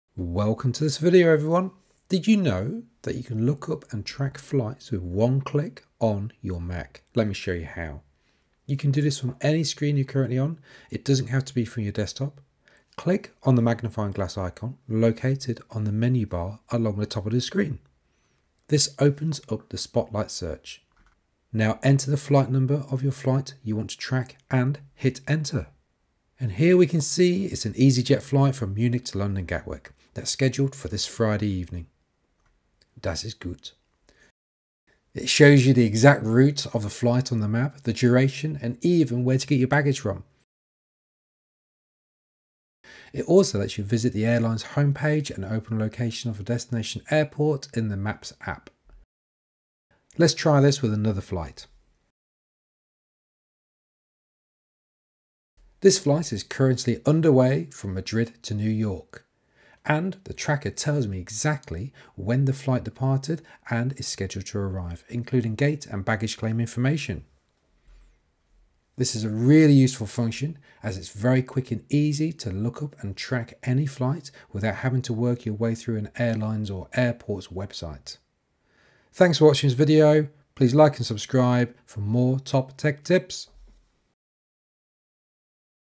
Apple's Flight Tracker - works on Mac, iPhone and iPad!
Did you know that you can track flights on your Mac with ONE click?! This works on your iPhone and iPad as well... try it now!